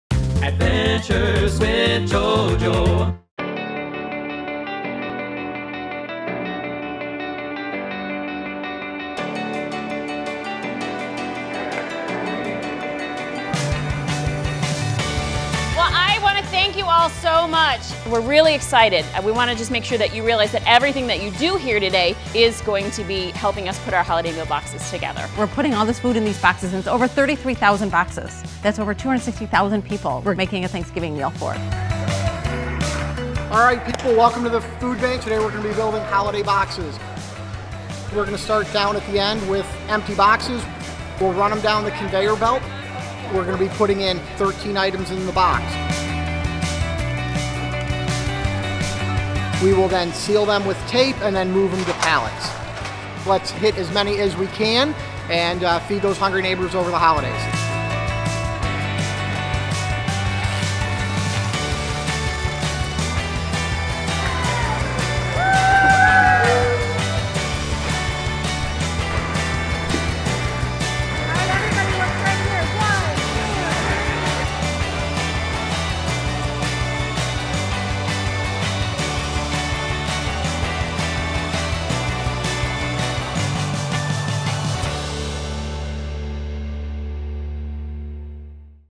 Adventures with JoJo: Volunteer Day!
JoJo helps Jewel-Osco associates pack items at the Northern Illinois Food Bank!